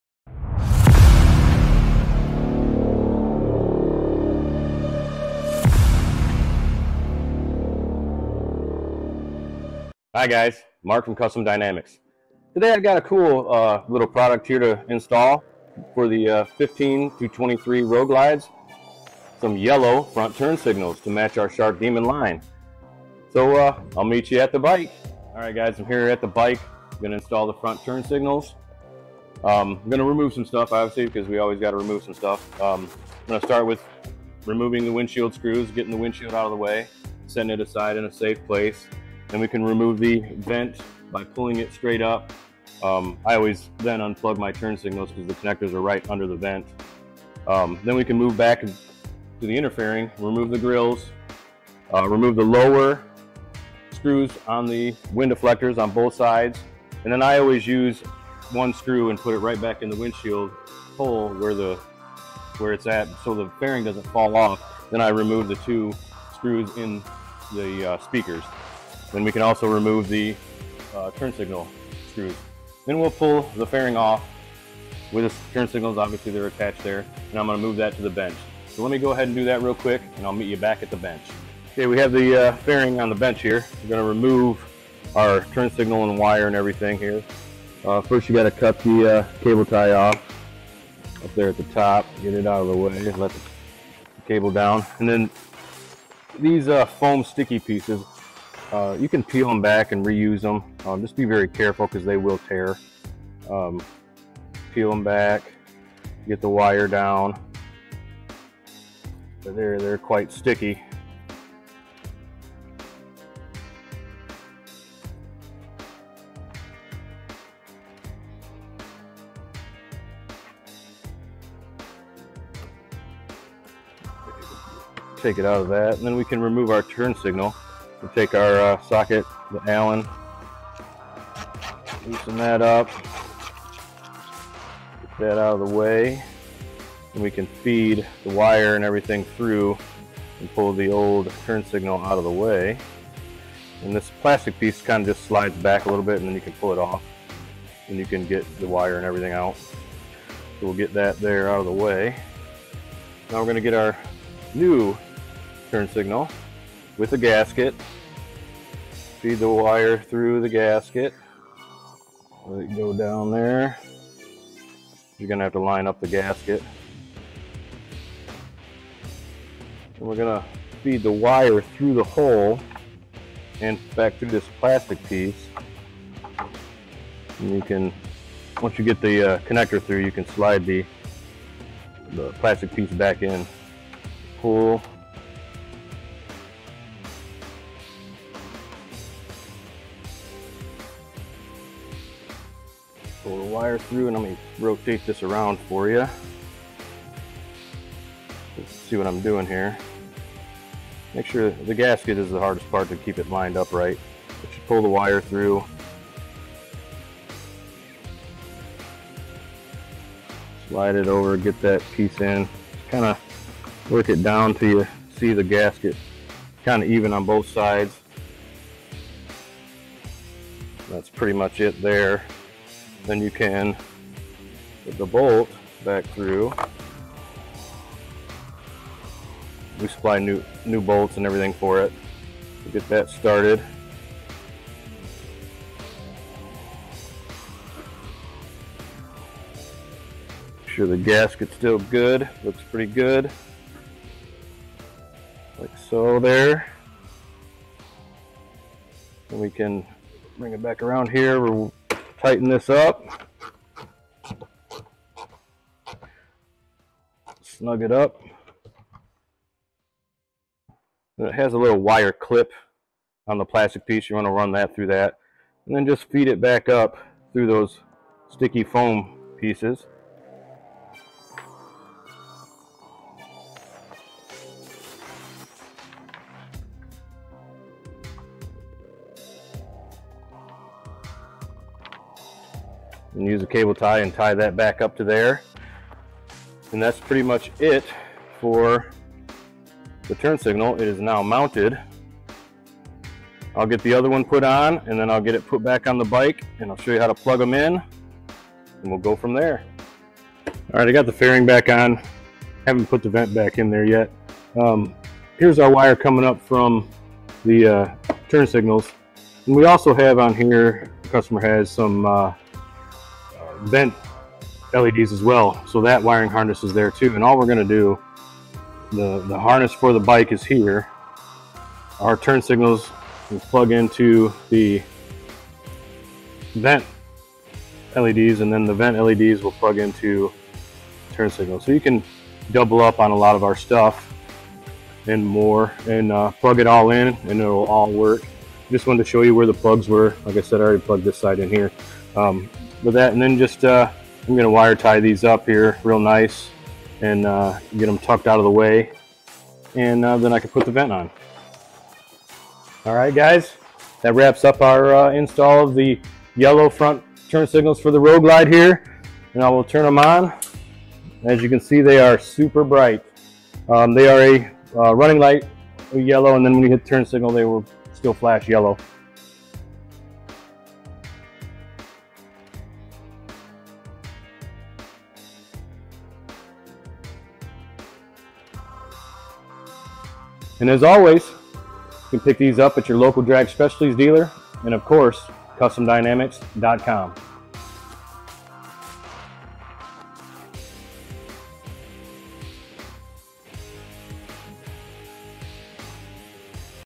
Transform Your Road Glide with Selective Yellow LED Motorcycle Front Turn Signals! 🏍️✨
High-performance LED front turn signals for your Harley-Davidson Road Glide motorcycle offer impressive brightness and clarity with SAE Selective Yellow running light and DOT compliant turn signal.

Mark walks you through the effortless installation using OEM turn signal wiring with plug and play connectors. Designed for on-road use and motorcycle applications, say good-bye 👋 to extra wire harnesses, relays, and separate brackets.